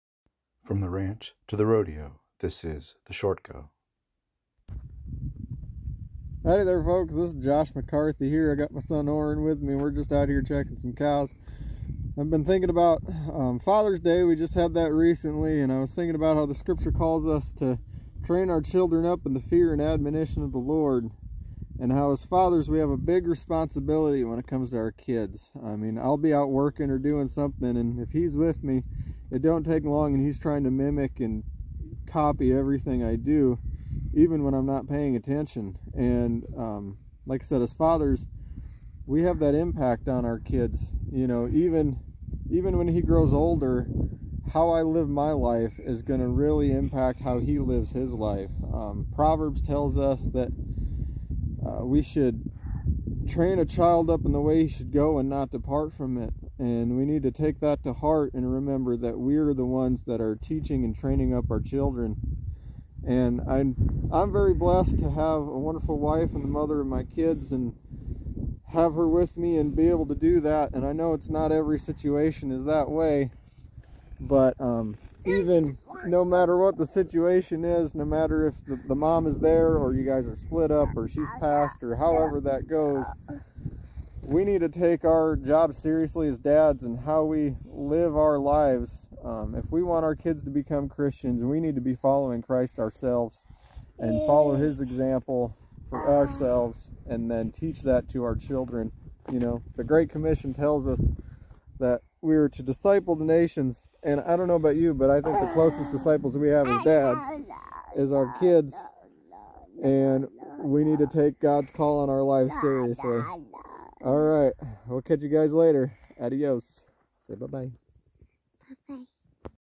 The Short Go: training up a child
What does it look like to "train up a child"? The Short Go with Idaho ranch hand Josh McCarthy and Orrin!